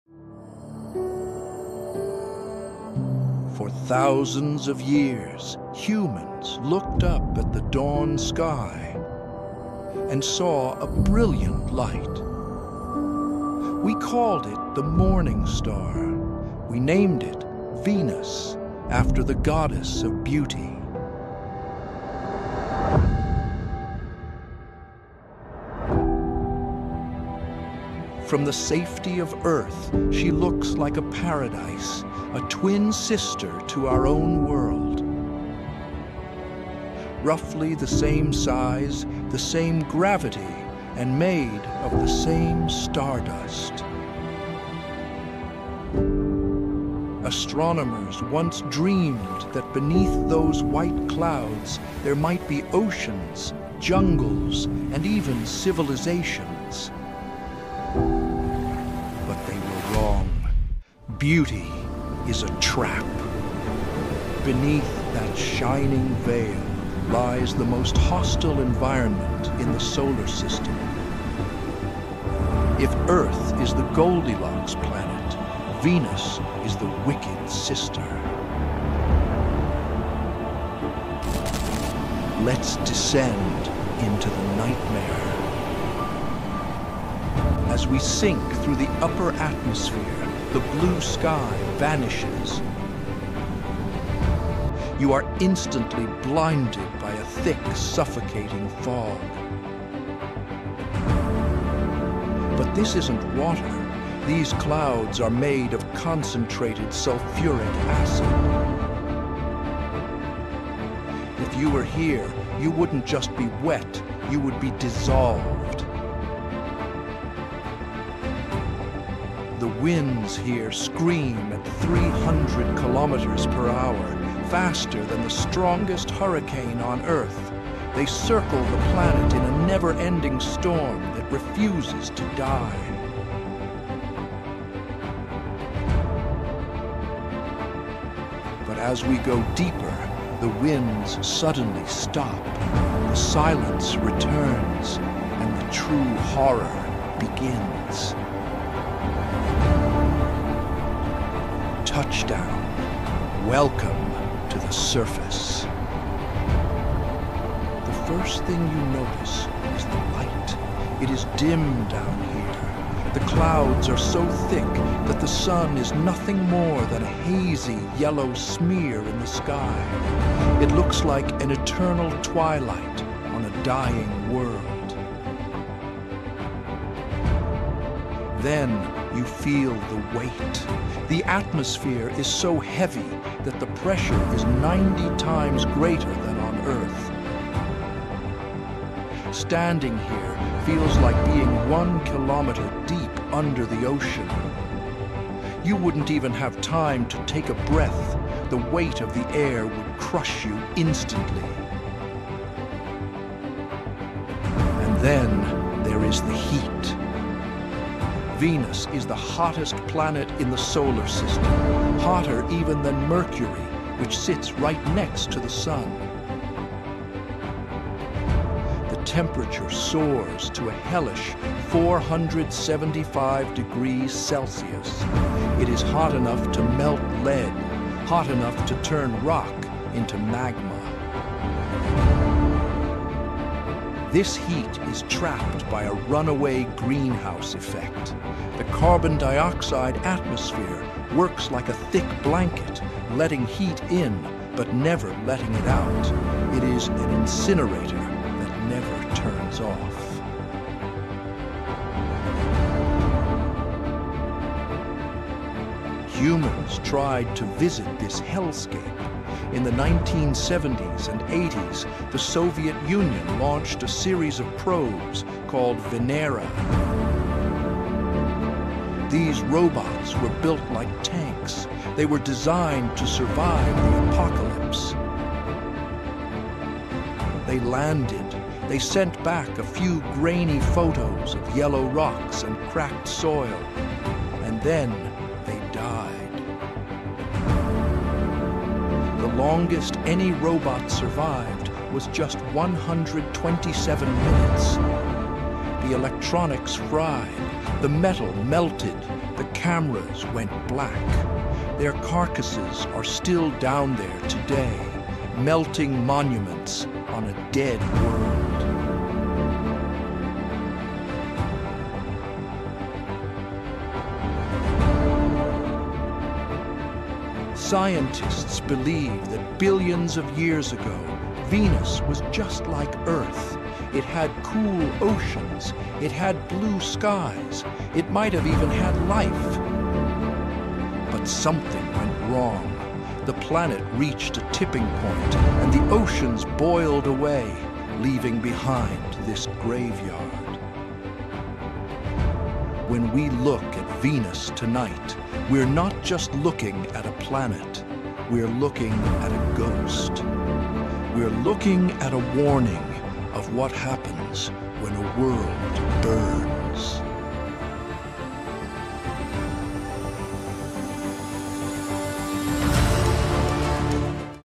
VENUS: The Real Surface is a Nightmare
We often look at Venus as Earth's beautiful twin sister. But beneath its thick clouds, the reality is a literal nightmare.

In this episode of Cosmic Lens, we peel back the layers of the "Morning Star" to reveal the true surface of Venus—a world where the air pressure crushes metal, the rain is made of acid, and the temperature is hot enough to melt lead.

We also revisit the chilling fate of the Soviet Venera probes, the only machines to ever capture images of this hellscape before they melted and died.

✨ Visuals created using SpaceEngine & Archival Data.